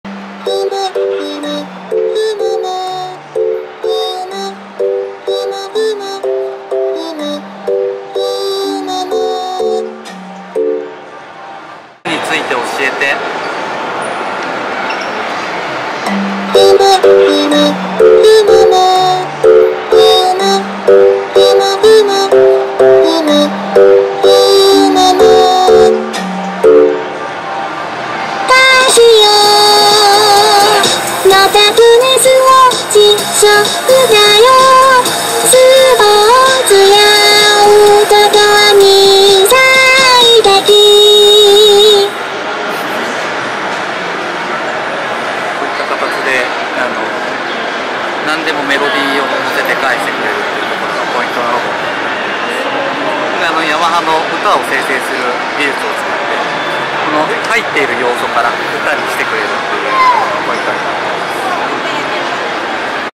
ヤマハの会話ロボットのチャーリー - Japan Mobility Show 2023
Japan Mobility Show 2023で展示されていた、ヤマハの会話ロボットのチャーリー。ヤマハの歌生成AIとOpenAIのChatGPTを組み合わせて、何でもメロディに乗せて会話をする。